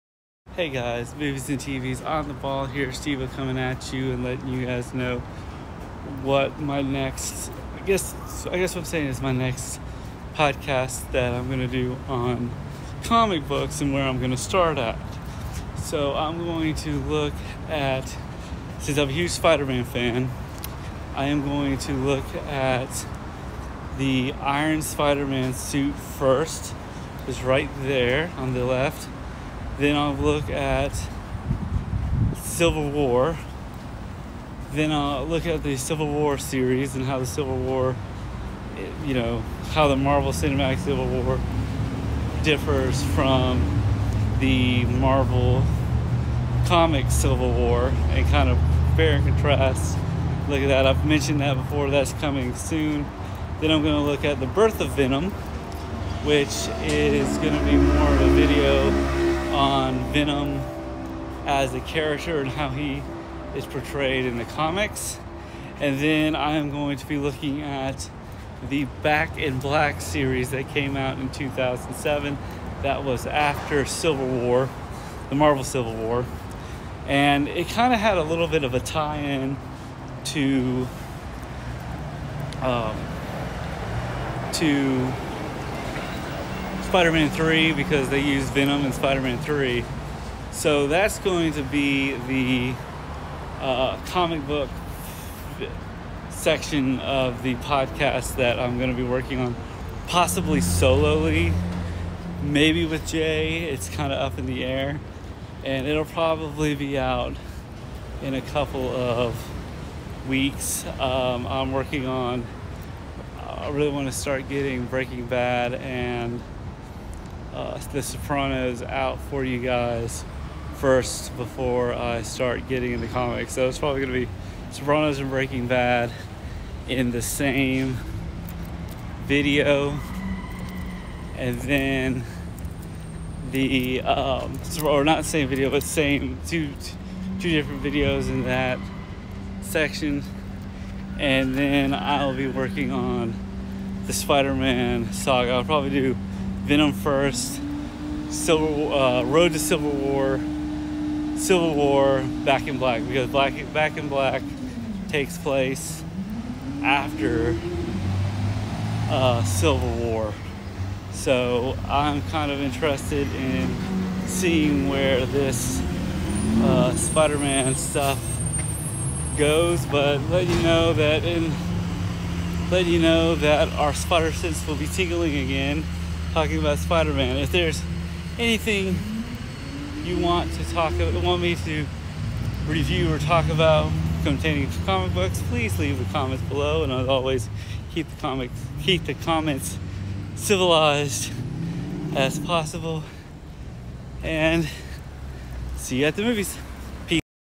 The ironspiderman Civil War back in Black with venom
Hey guys, just to let you know And going into comic books. I’ll be covering Venom BlackSuitSpider-Man.